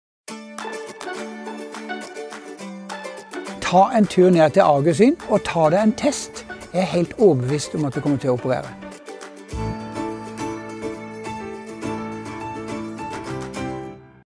Gjør som Dag Otto, bli kvitt briller og kontaktlinser!
Den synskorrigerende linseoperasjonen til Dag Otto Lauritzen gikk raskt og smertefritt og han ser nå klart og godt uten briller og kontaktlinser.

Dag Otto angrer bare på én ting - at han ikke fikk gjort dette for lenge siden.

Lytt derfor til rådet fra Dag Otto. Ta kontakt med Argus Syn i dag! 😊